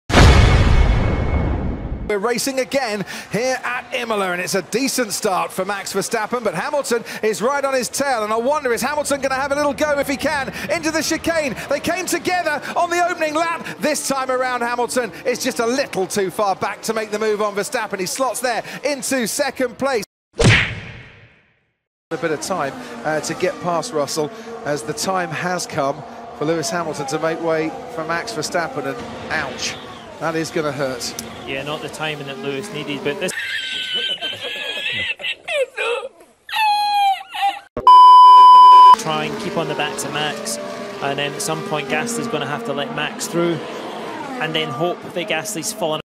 Lewis Hamilton vs. Max Verstappen (2021&2022 Imola)
The comparison of their battles against each other at Imola.